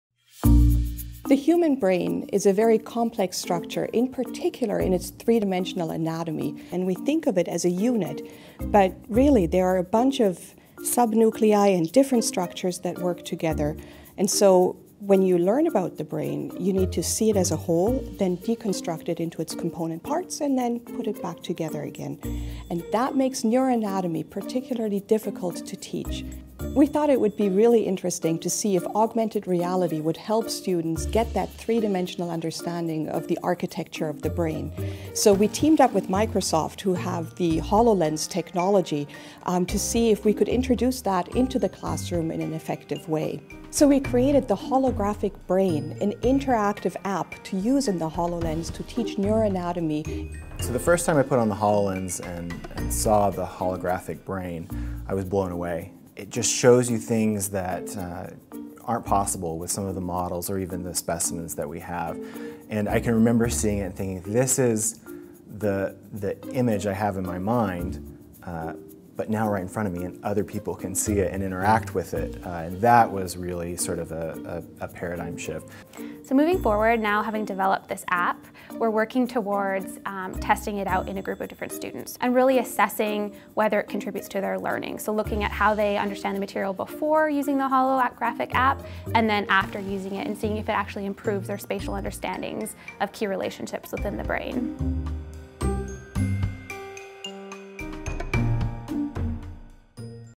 Mixed reality gives neuroanatomy lessons a boost
With its unique hemispheres and intricate connections, the brain is one of the most exceptional, yet complex organs in the human body. The intricate structure has posed a unique challenge for generations of students looking to understand and ultimately master how its many parts fit, and work, together.

But this fall — thanks to Microsoft HoloLens and mixed reality technology — UBC students will get to visualize the brain’s 3D structure in a way they never have before.